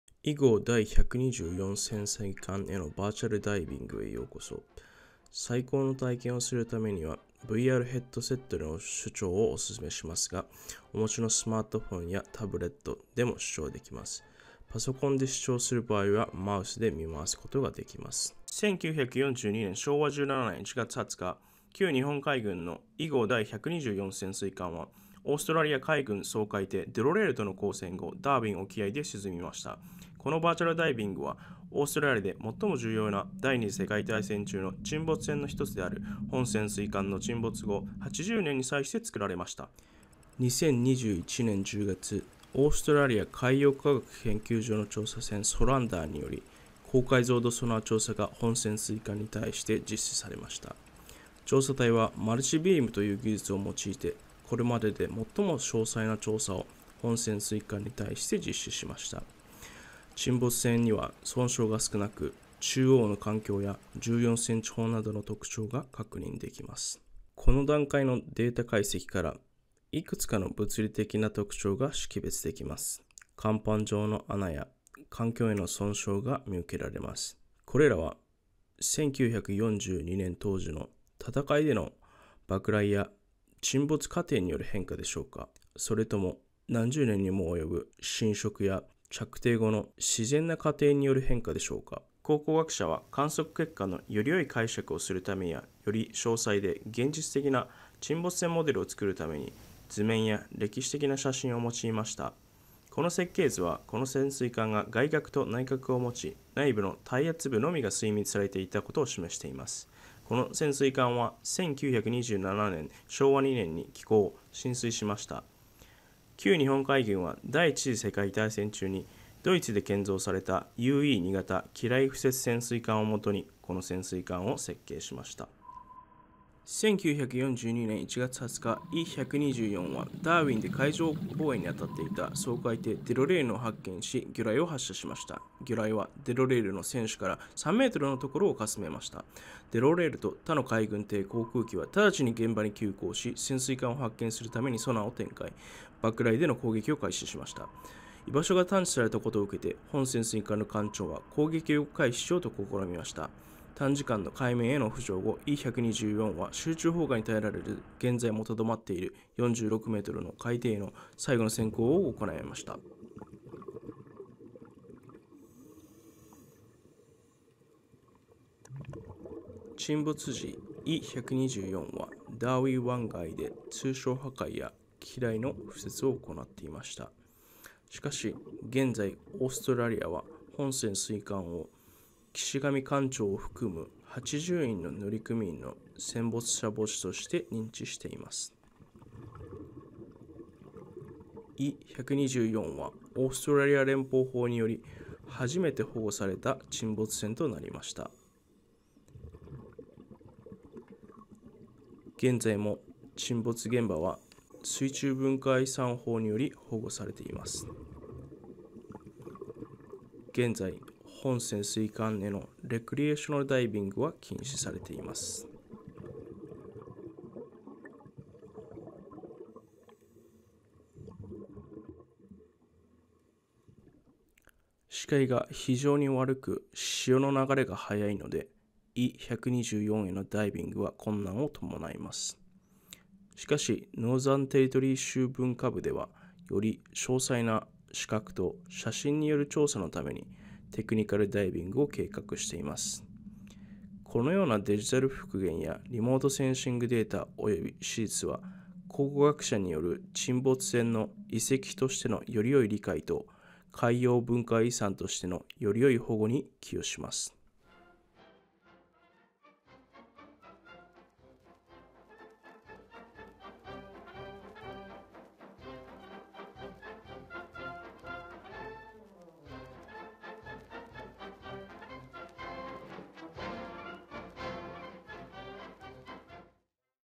旧日本海軍伊１２４潜水艦へのバーチャルダイビング（日本語版）
旧日本海軍伊１２４潜水艦は、1942年1月20日にオーストラリアのダーウィン沖で沈没しました。このVR体験は、オーストラリア海洋科学研究所が2021年10月に実施した最新のマルチビームソナーの調査結果で始まり、潜水艦沈没過程のアニメーション、設計を理解する為の色々な情報源、そして沈没後の数年間に出現した可能性がある甲板のバーチャルダイビングで終了します。このジョン・マッカーシーによる調査と視覚化は、豪州北部準州文化部から委託されました。文化部では今後も潜水艦の状態を確認する為に、沈没現場でのダイビング調査を計画しています。

この遺跡の詳細については、以下を参照してください（英語）

北部準州豪日協会による潜水艦とその記念碑の専用ページ（英語）

ダーウィンを拠点とする歴史家トム・ルイス博士による伊１２４潜水艦の詳細な記述をされた優れた書籍も参照してください（英語）
このビデオの英語版は本チャンネルから視聴可能です。